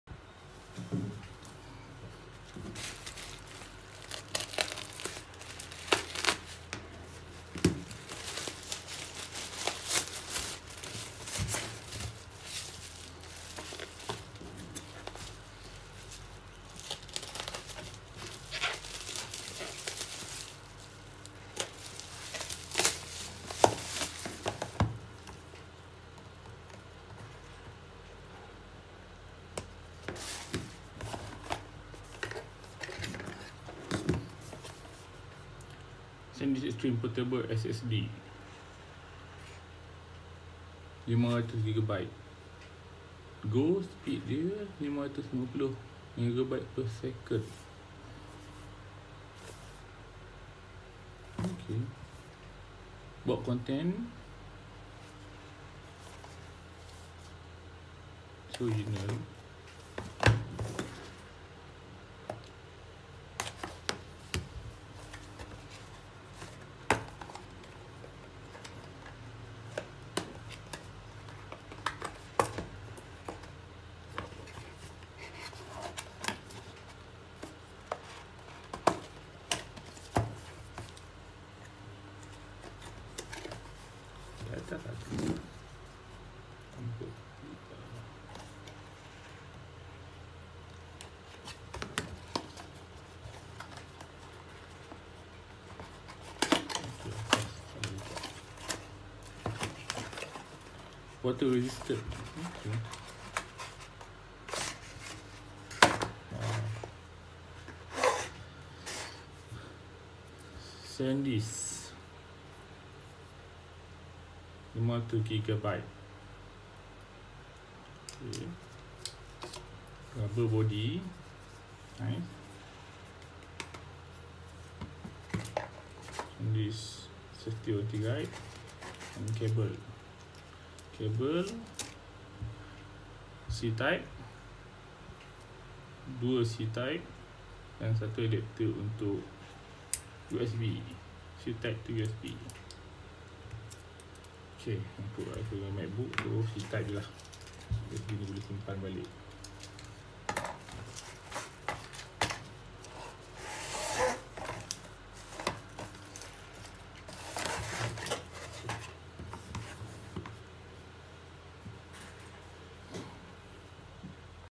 Unboxing SanDisk Extreme Portable SSD
Nih n compact.. what 1 bought is 500GB  external SSD.. this model come with 250GB, 500GB, 1TB & 2TB 🥰